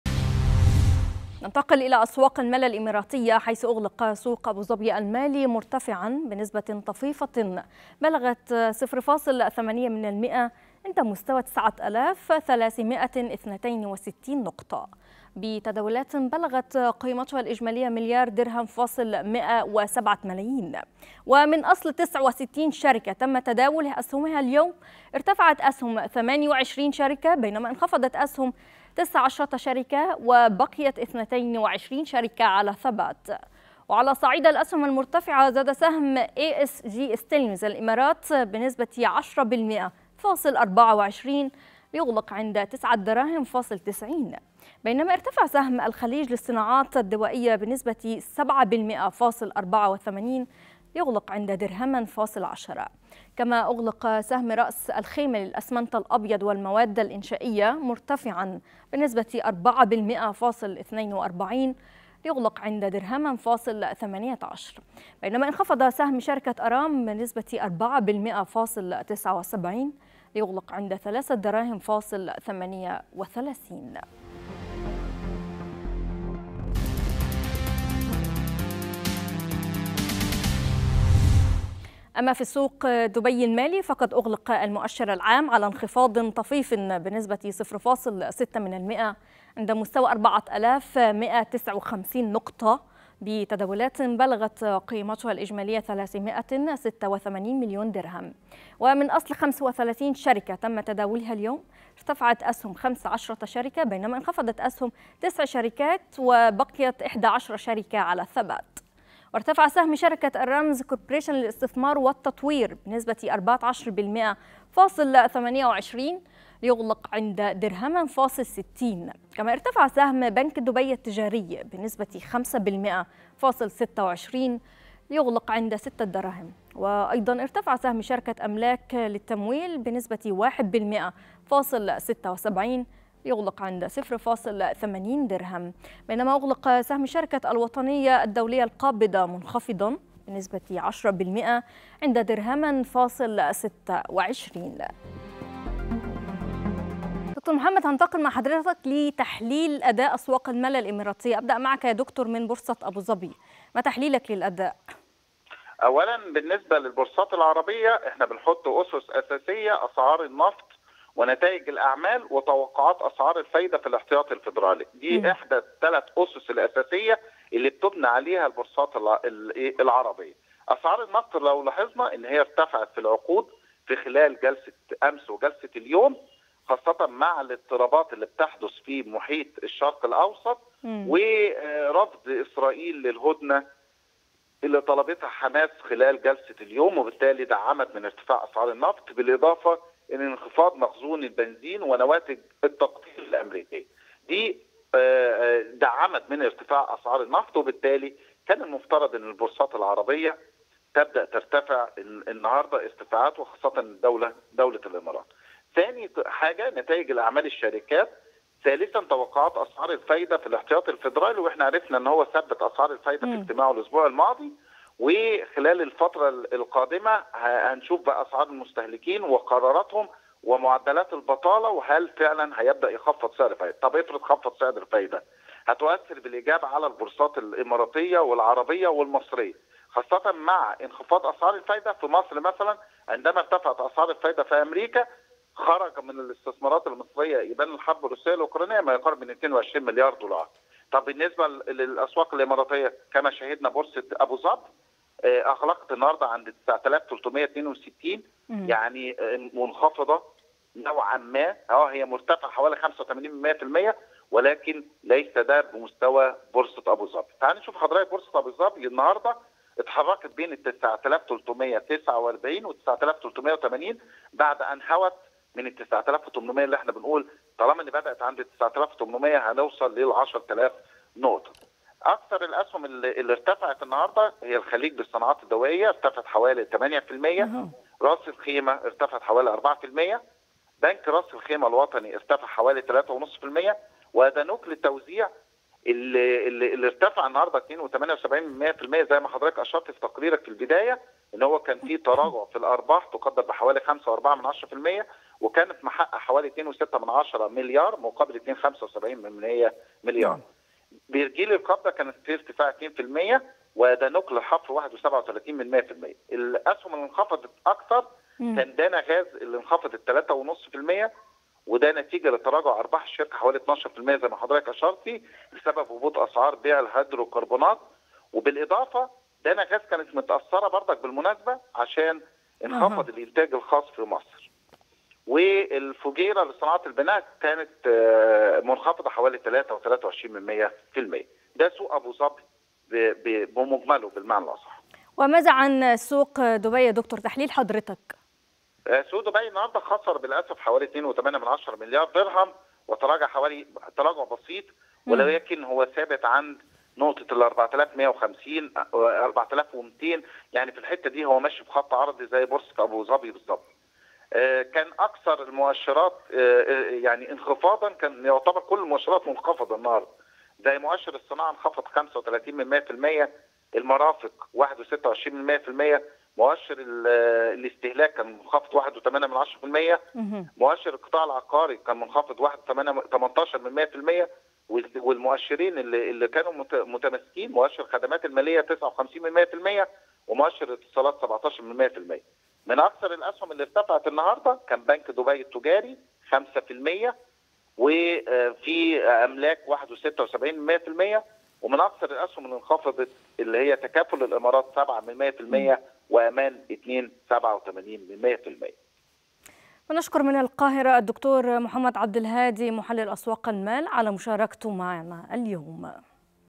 مؤشرات وتحليل أبرز ما جاء فى سوق المال الاماراتي 🇦🇪 مع د / محمد عبد الهادي 8/2/2024
أرقام وأسواق : اللون الأخضر يسيطر على أسواق المال العربية ختام تعاملات الخميس
تحالف إماراتي يستثمر اثنين وعشرين مليار دولار في أضخم مشروع سياحي بمصر
النفط يرتفع مدعوما بتفاقم توترات الشرق الأوسط
هاتفيا : الدكتور / محمد عبد الهادي .. محلل أسواق المال
8/2/2024